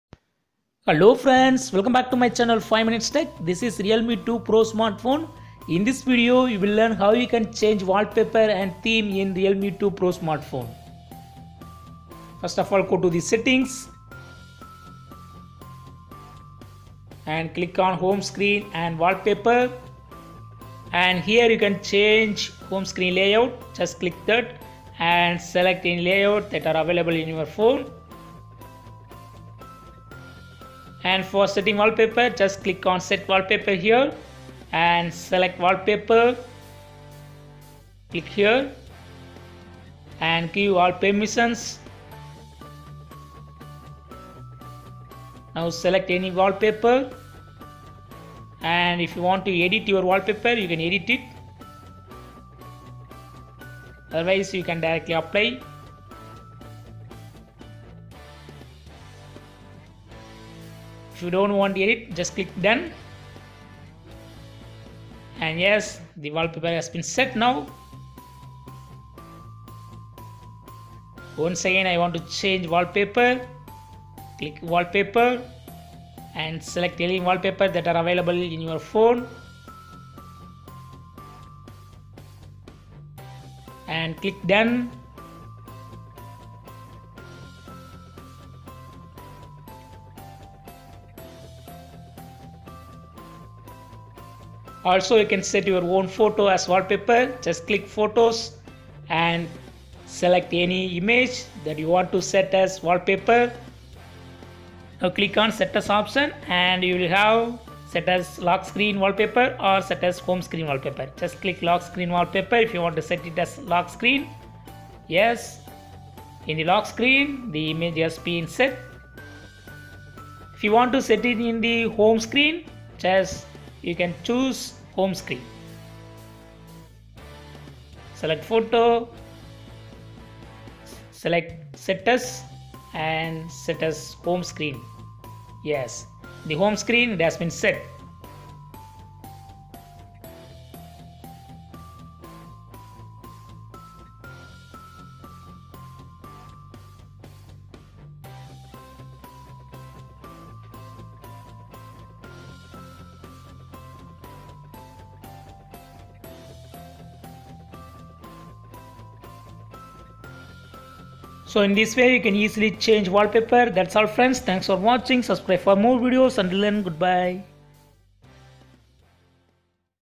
How to change wallpapers in RealMe 2 Pro
RealMe 2 Pro wallpaper change settings: Go to settings in your RealMe 2 Pro smartphone. Now select lock screen and wallpapers. Now you have to select set Wallpaper. Here you will have so many Wallpaper that are readily available to use. Choose any one and click on apply Button. You will be asked to edit your image if you want. Otherwise just click done without any edit on the wallpaper and it will be set as home screen as well as lock screen wallpaper. You can also set your own photos as Wallpaper. For that select photos option after clicking set as wallpaper option. You have to choose any photo that you have in your gallery. You can now select set as lock screen wallpaper or set as home screen wallpaper to set in the background of your RealMe 2 Pro smartphone. That's all... Enjoy changing wallpapers as per your wish in your RealMe 2 Pro smartphone.